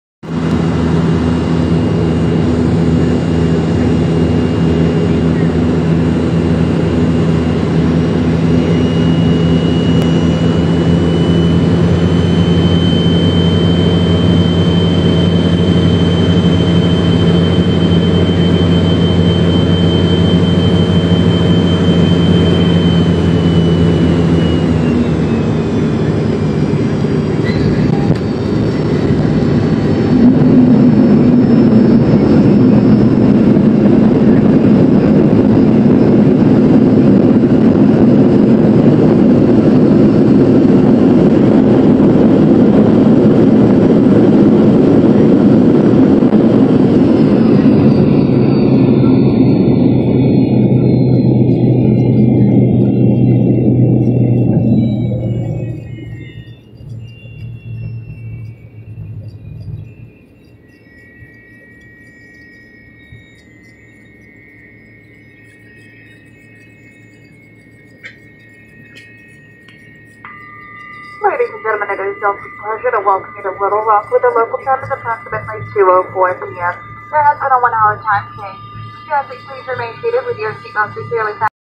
Delta MD-88 landing in Little Rock, Arkansas
Flight from ATL landing over the Arkansas River and AR became my 41st state to visit 2/23/18.